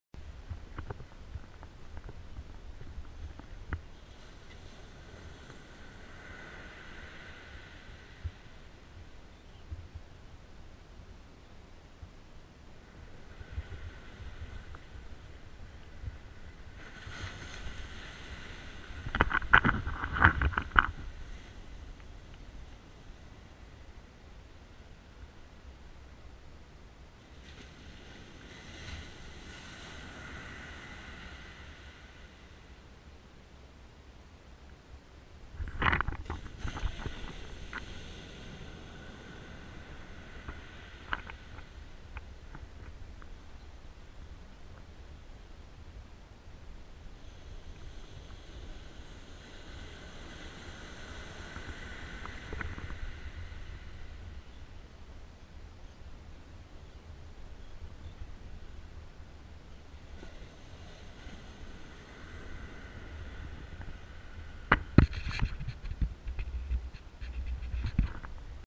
My Movie Sea Turtle Hatch
While fishing at Ponce Deleon Park , Central Florida , I noticed some Sea Turtles hatching and took a short video of the event !     ( 10/29/15 )